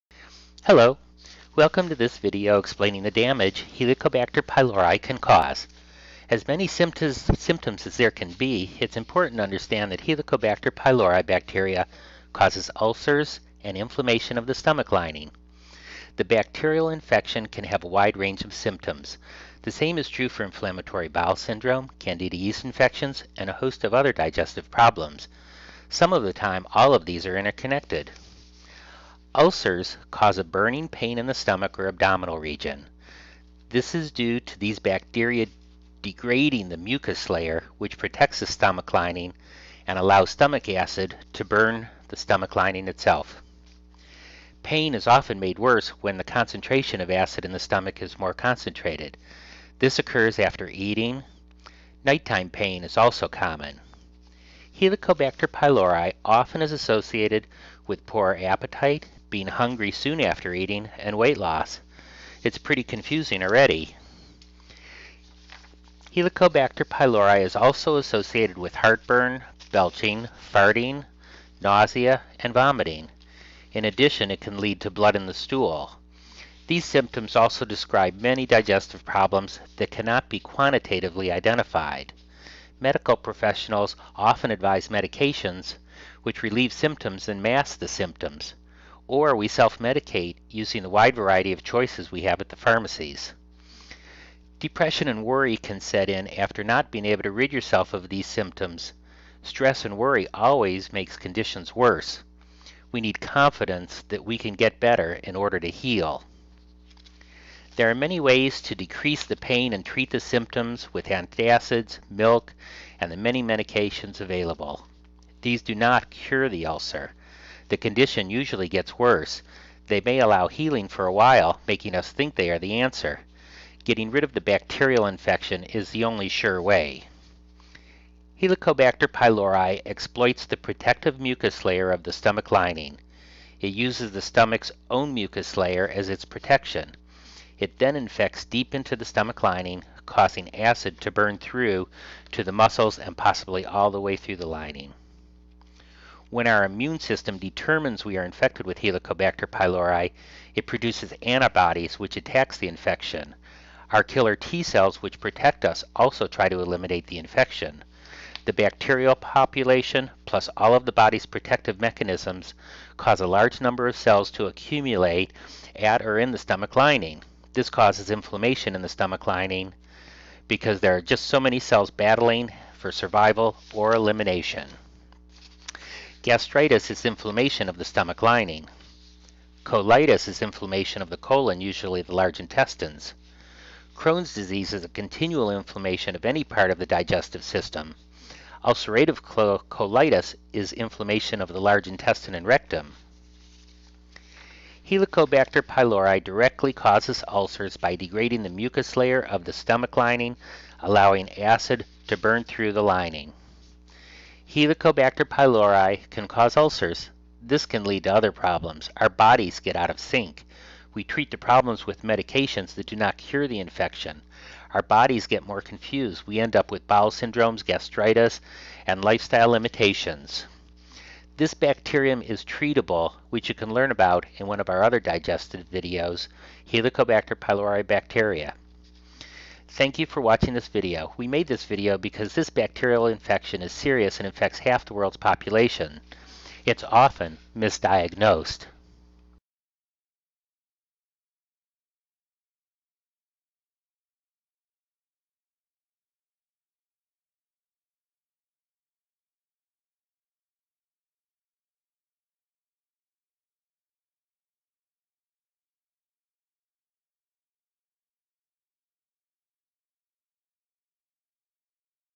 Helicobacter pylori Damage explanation and diagrams
Uploaded on Jan 3, 2012
26 1st 4 slides Hello, welcome to this video explaining the damage Helicobacter pylori can cause. As many symptoms as there can be, it is important to understand that helicobacter pylori bacteria causes ulcers and inflammation of the stomach lining. This bacterial infection can have a wide range of symptoms. The same is true for Inflammatory Bowel Syndrome, Candida Yeast Infections, and a host of other digestive problems. Many times, all of these are interconnected.
8 Ulcers cause a burning pain in the stomach or abdominal region. This is due to this bacteria degrading the mucous layer which protects the stomach lining allowing stomach acid to burn the stomach lining.
10 Pain is often made worse when the concentration of acid in the stomach is stronger. This occurs after eating. Nighttime pain is also common.
10 Helicobacter pylori often is associated with poor appetite, being hungry soon after eating, and weight loss. See how it gets confusing already?
7 Helicobacter pylori is also associated with heartburn, belching, farting, nausea, and vomiting. In addition it can lead to blood in the stool. These symptoms also describe many digestive problems that can not be quantitatively identified. Medical professionals often advise medications which mask the symptoms. Or we self medicate using the wide variety of choices we have at the pharmacies.
9 Depression and worry can set in after not being able to rid yourself of these symptoms. Stress and worry always makes conditions worse. We need confidence we can get better in order to heal.
9 There are many ways to decrease the pain and treat the symptoms with antacids, milk, or the many medications available. These do not in any way cure the ulcer. The condition usually gets worse. They may allow healing for a while making us think they are the answer. Getting rid of the bacterial infection is the only sure way.
15 Helicobacter pylori exploits the protective mucous layer of the stomach lining. It uses the stomach's own mucous layer as its protection. It then infects deep into the stomach lining causing acid to burn through to the muscles and possibly all the way through the lining.
12.5 2 slides When our immune system determines we are infected with Helicobacter pylori, it produces antibodies which attacks the infection. Our killer T-Cells which protect us also try to eliminate the infection. The bacterial population, plus all of the body's protective mechanisms cause a large number of cells to accumulate in the stomach lining. This cause inflammation in the stomach lining due to all the cells battling for survival or elimination.
5 Gastritis is inflammation of the stomach lining.
5 Colitis, is inflammation of the colon, usually the large intestine.
5 Crohn's is continual inflammation of any part of the digestive system.
Ulcerative Colitis is inflammation of the large intestine and rectum.
9 Helicobacter pylori directly causes ulcers by degrading eh mucous layer of the stomach lining allowing acid to burn through the lining.
20 Helicobacter pylori can cause ulcers. This can lead to other problems. Our bodies get out of sync. We treat the problems with medications that do not cure the infection. Our bodies get more confused. We end up with bowel syndromes, gastritis, and lifestyle limitations.
This bacteria is treatable which you can learn about in one of our other digestion Helicobacter Pylori Bacteria video.
We made this video because this bacterial infection is serious and infects half the world's population. It is often misdiagnosed.
Category:
Education